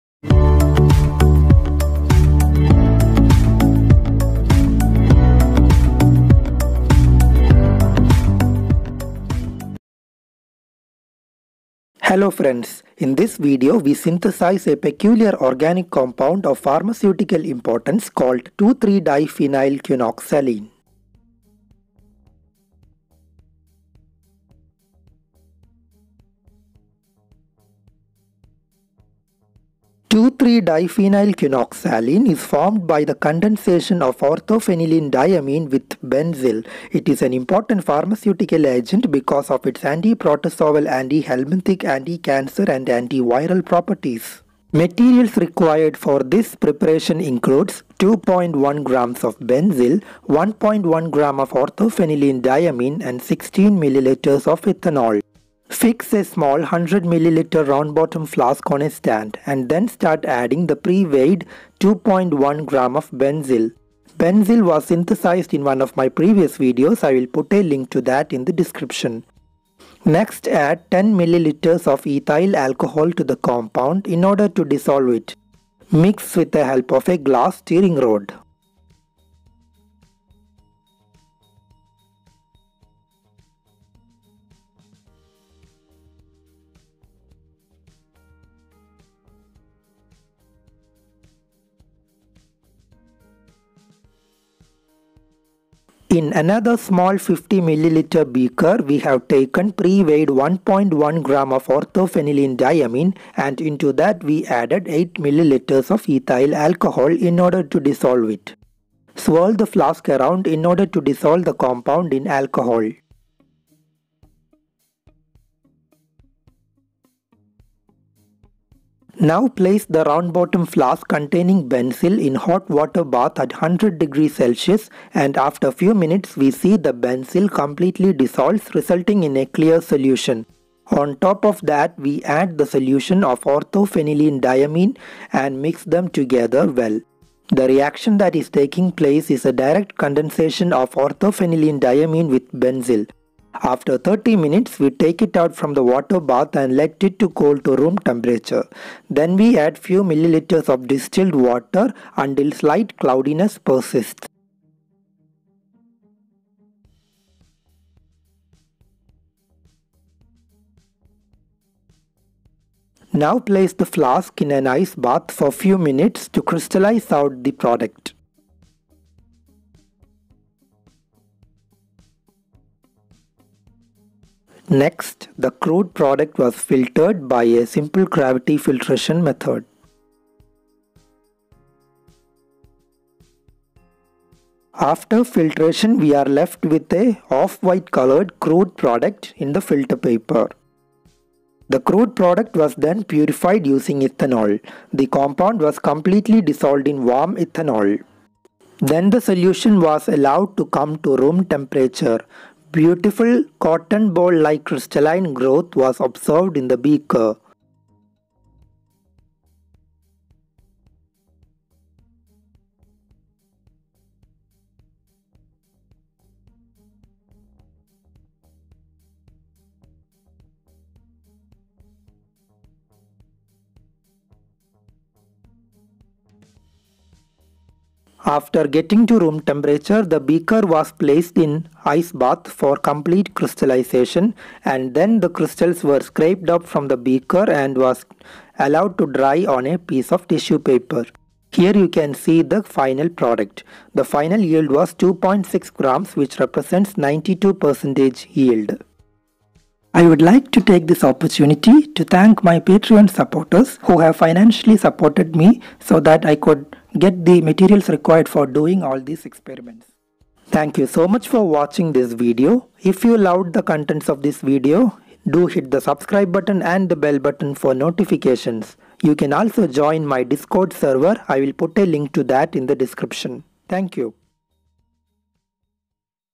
2,3 Diphenylquinoxaline : Organic synthesis
Hello friends in this video we will synthesise 2,3 diphenylquinoxaline by the direct condensation of o-phenylenediamine with benzil

be safe in home during this pandemic season and try to watch all my videos.

This video has been recorded and edited with much effort and difficulty. Lack of proper equipment and chemicals can make video look unfinished.

Or